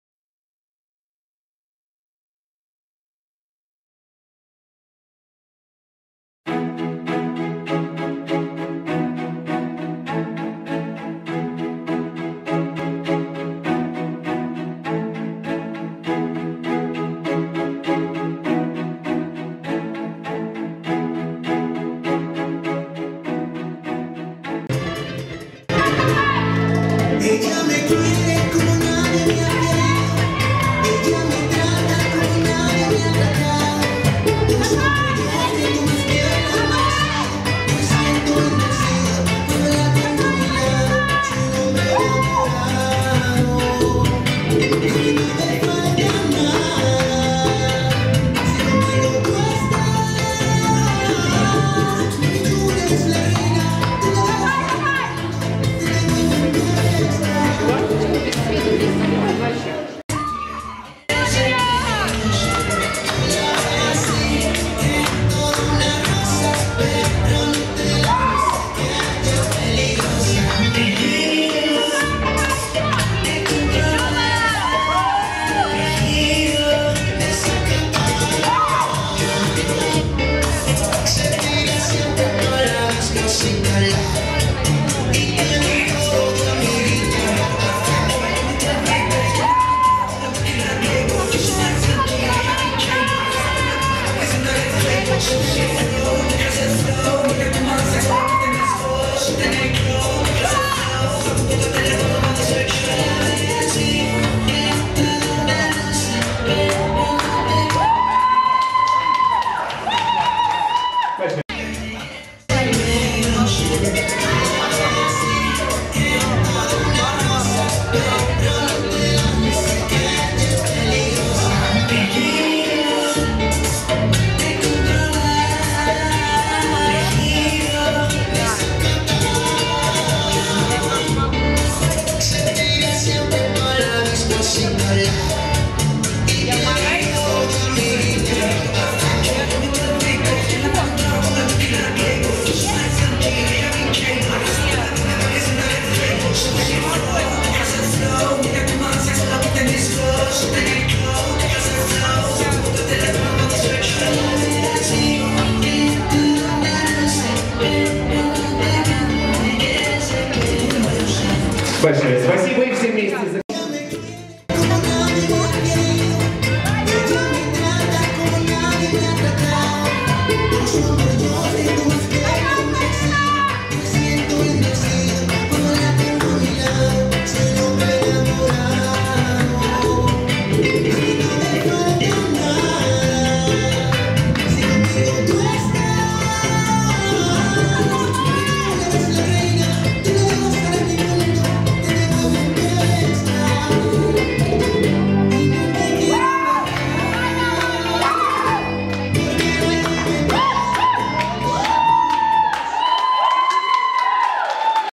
BACHATA (SF) Artistic Dance Awards 2022
Artistic Dance Awards 2022
Radisson Collection Hotel
(Bachata Youth+Adults Solo Female Semifinal)
Отель «Radisson Collection»
(Бачата Молодежь+Взрослые  Соло Девушки Полуфинал)